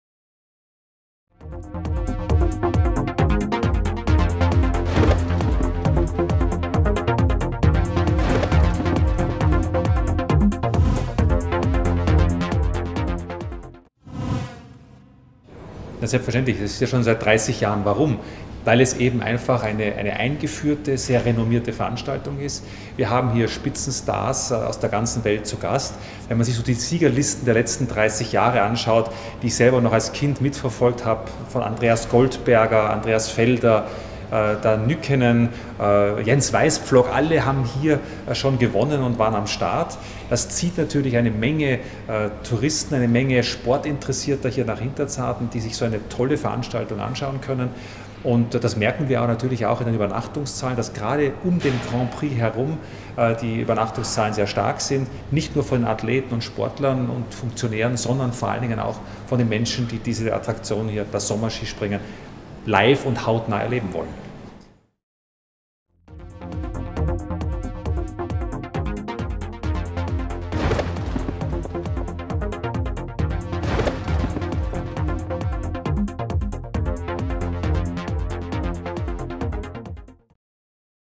Sommerskispringen in Hinterzarten 2011: Thorsten Rudolph
Thorsten Rudolph, Geschäftsführer der Hochschwarzwald Tourismus GmbH, über die Bedeutung des Sommerskispringens für HInterzarten.